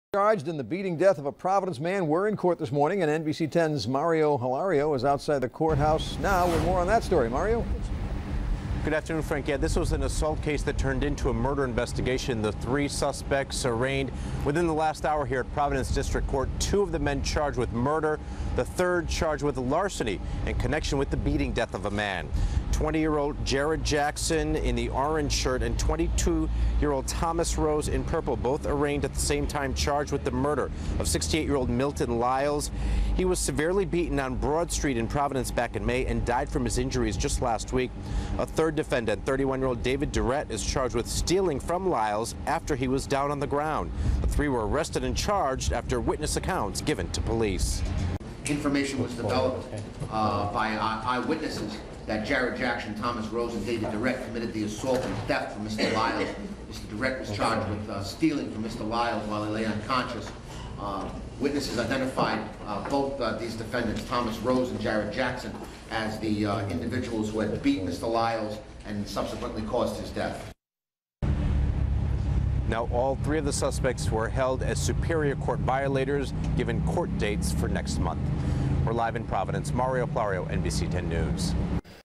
Men arraigned in Providence beating death
Two of the men were arraigned and charged with murder, the other was charged with larceny, all in connection with the beating death of a man.